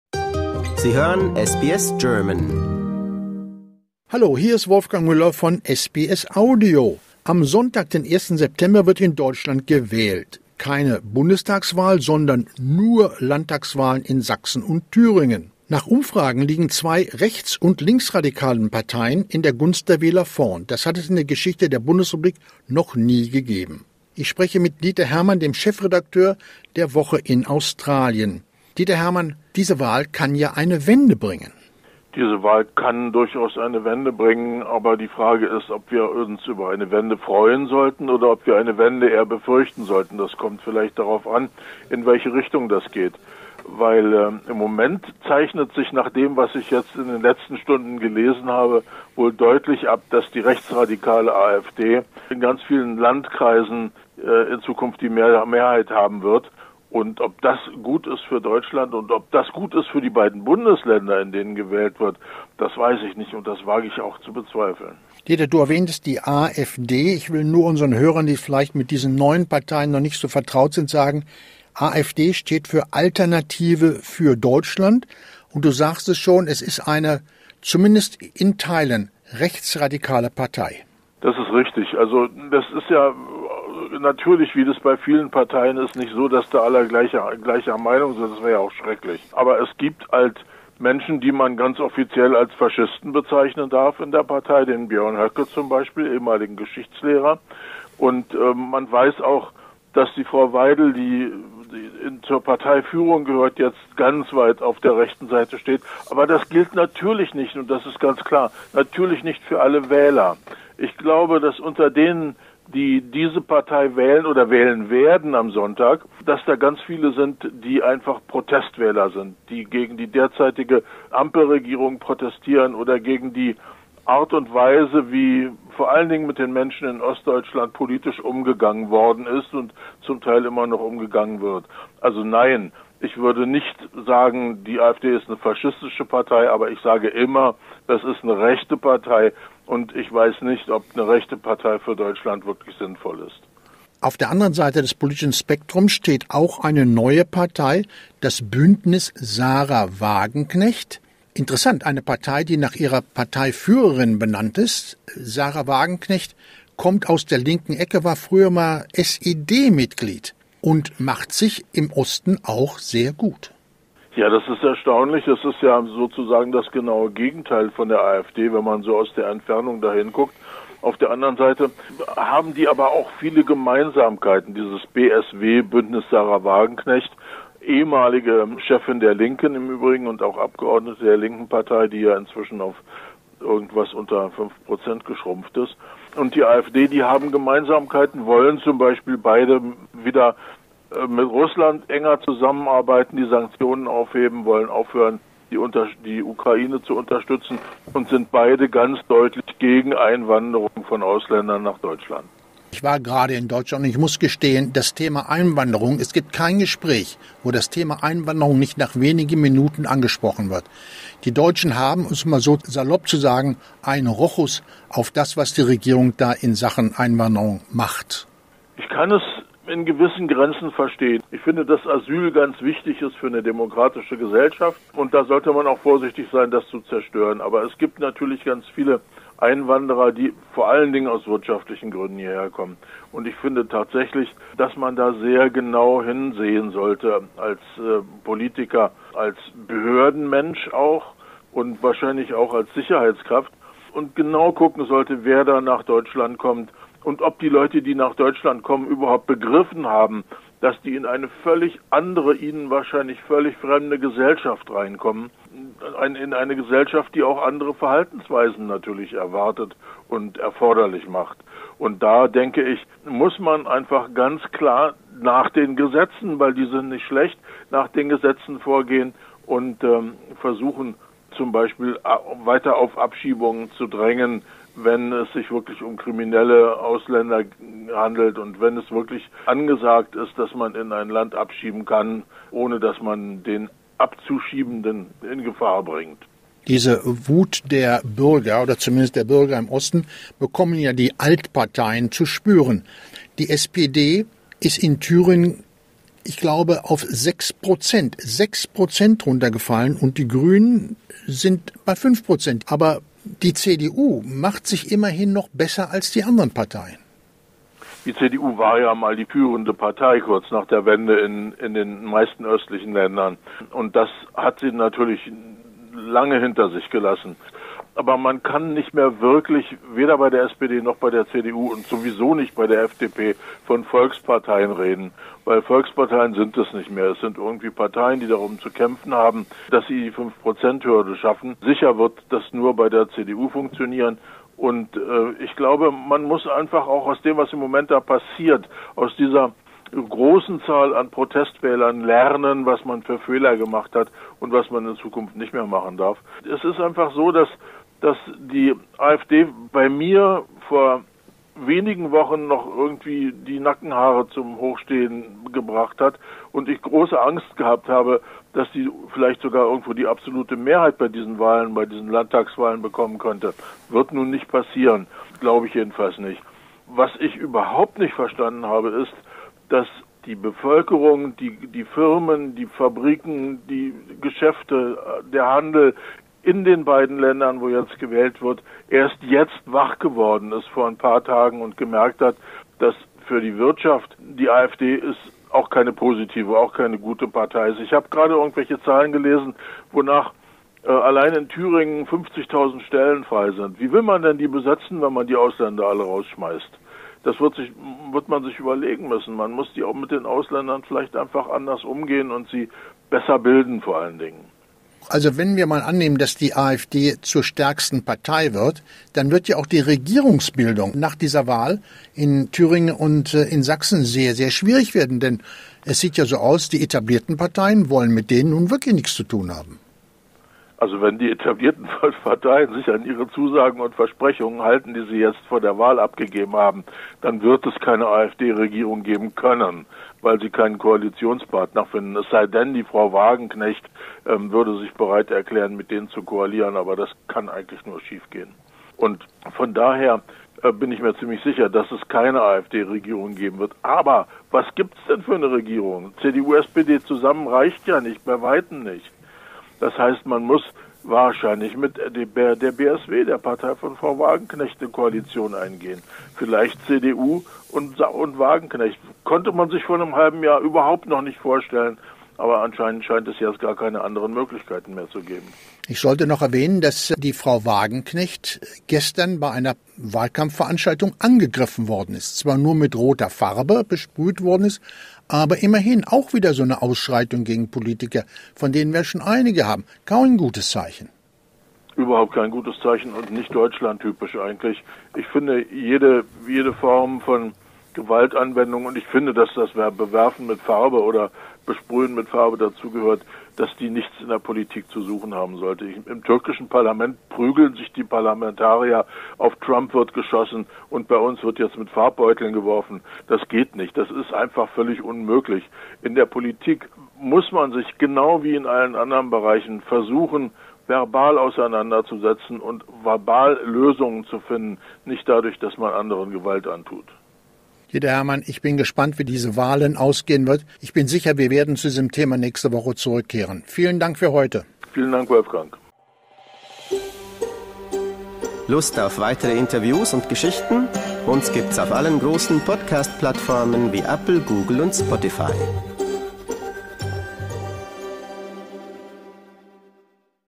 Triumph of radical parties? - Triumph radikaler Parteien? | SBS German - SBS Deutsch
According to surveys, two far-right and left-wing parties are ahead in the state elections in Saxony and Thuringia. Are there signs of a profound political change in Germany? Dieter Herrmann, editor-in-chief of WOCHE IN AUSTRALIA, answers this question. - Nach Umfragen liegen bei den Landtagswahlen in Sachsen und Thüringen zwei rechts- und linksradikale Parteien in der Gunst der Wähler vorn. Zeichnet sich eine tiefe politische Veränderung in Deutschland ab? Diese Frage beantwortet Dieter Herrmann, Chefredakteur der WOCHE IN AUSTRALIEN.